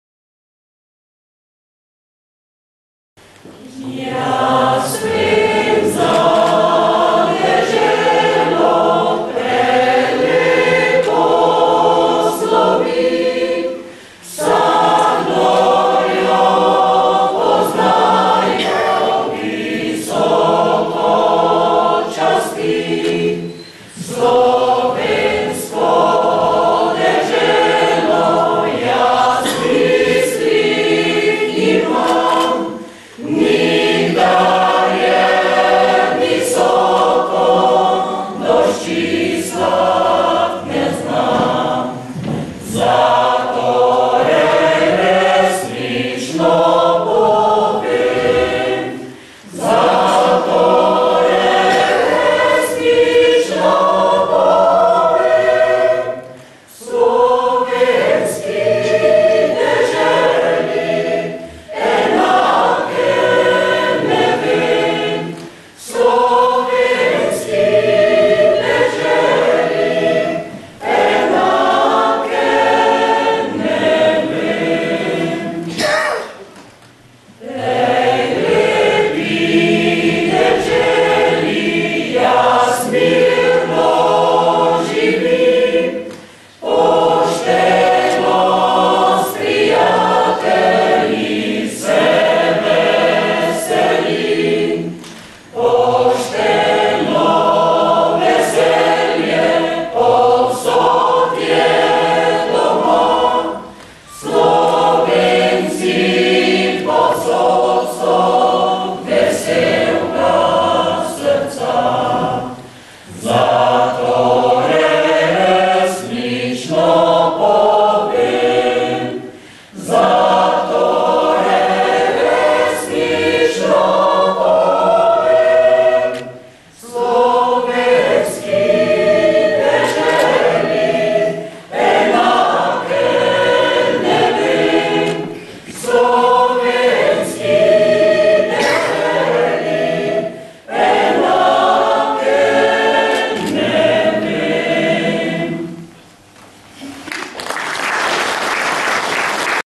MePZ Dobje
Na prireditvi ob 22. juliju, prazniku Občine Dobje